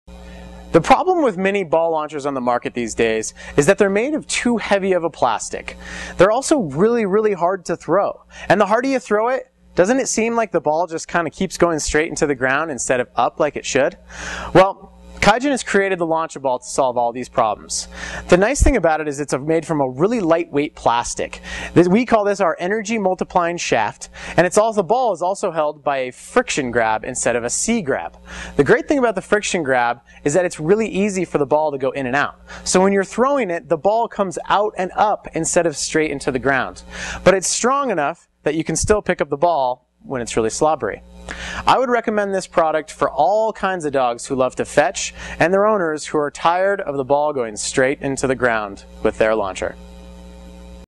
Dog Games - Launch A Ball
Energy multiplying shaft and friction-based release!

The Launch-a-Ball is the most advanced innovation of its kind, and offers dog owners the opportunity to send a ball sailing, regardless of their natural throwing ability. The shaft is specifically designed to store energy before release. The tennis ball is held in place by a friction grip head. No more launching straight into the ground like the competitors' snap fit!